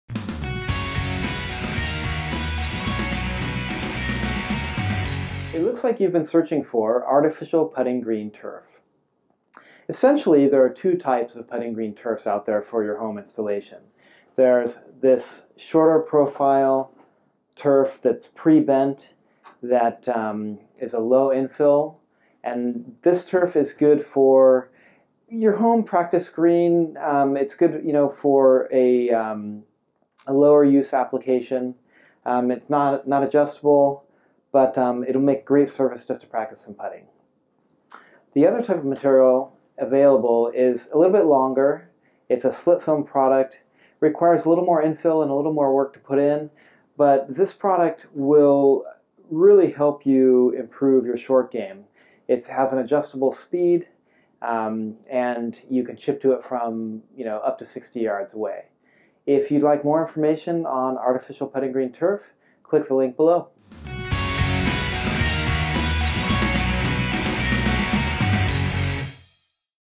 ATE Searches – Artificial Putting Green Turf
This is a video that we have created with the exact purpose of informing you about "artificial putting green turf." There is a lot to know about "artificial putting green turf," and we have a lot of experience with installing putting greens with artificial turf, so it is our hope that this experience can be beneficial to you. Many people do not realize that today's artificial turf is far more advanced than its astroturf predecessors of the 20th century.

After watching the video, you should know a lot more about artificial putting green turf.